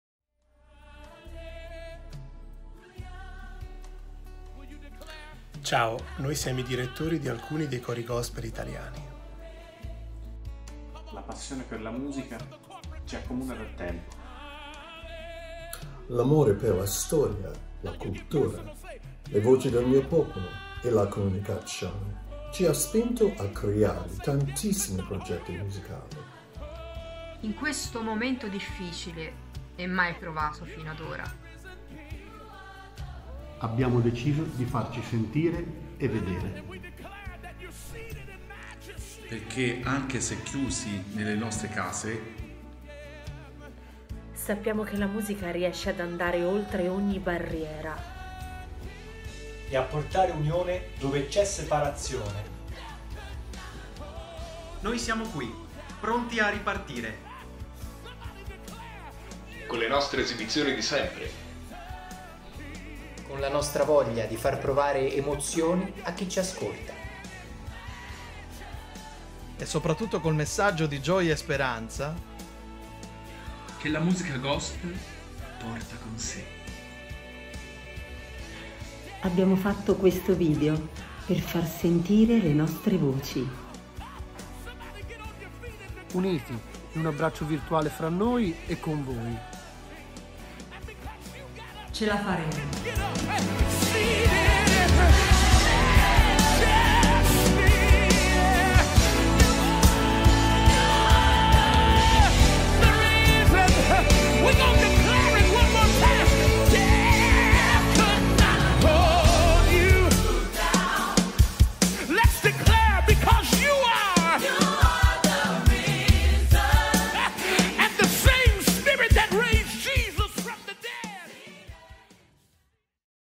Gospel Hug (AndràTuttoBene)
01 aprile 2020
I direttori di alcuni cori gospel italiani mandano il loro messaggio di unione, aspettando il ritorno ad una rinnovata normalità.

Giorgio Ammirabile - One Voice Gospel Choir
Marco Befani - For Joy Contemporary Gospel Choir
Nehemiah H. Brown - Florence Gospel Choir School
Patrizia Calussi - The Gospel Fire & Little Big Gospel Choir
Luca Crisafulli -
The Marching Saints Gospel Music

Pietro Paolo De Luca - Saint Paul Gospel Choir
Letizia Dei - Light Gospel Choir

Luca Del Tongo - Jubilation Gospel Choir
Federico Fiorentin -
New Generation Gospel Crew
Alessandro Gerini - Vocal Blue Trains
Stefano Giacomelli - Joyful Voices Gospel Choir
Luca Luglio - July Gospel Singers

Gianni Mini - The Pilgrims Gospel

Fausta Molfini - Peter’s Gospel Choir
Leandro Morganti - Prato Gospel School
Marina Taccola - Music Art Academy Gospel Team